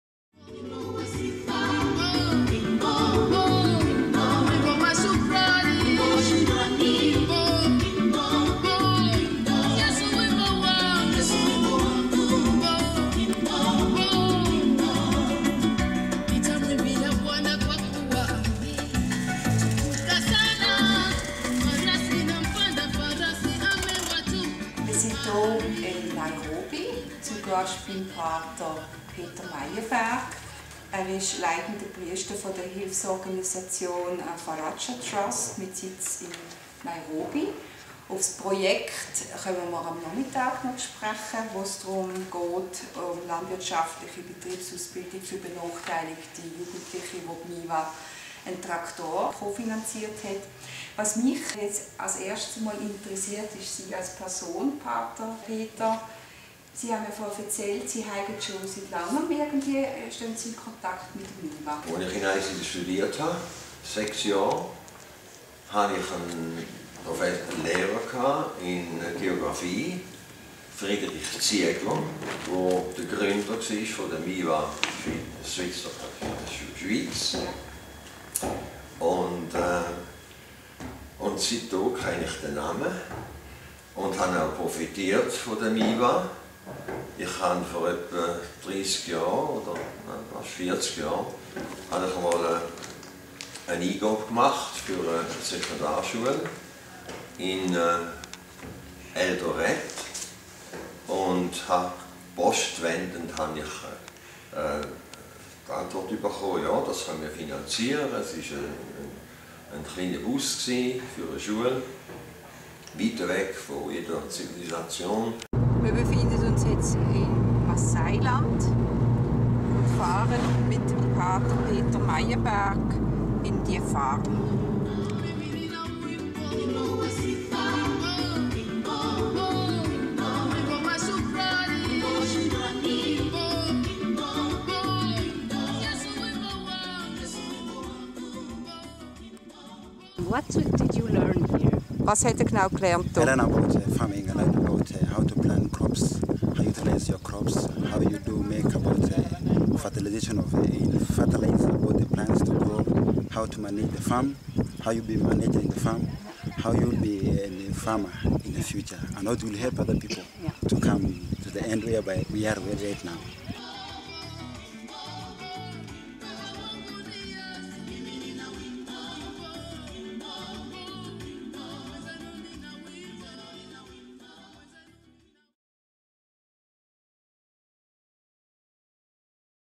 miva Projektreise Kenia 2013 - Besuch bei Pater Peter Meienberg
Pater Peter Meienberg ist langjähriger Leiter der Organisation Faradja Trust Nairobi. miva hat für ein Ausbildungsprojekt einen Traktor finanziert. Auf der Farm der Faradja Trust werden benachteiligte Jugendliche im bereich Landwirtschaft ausgebildet.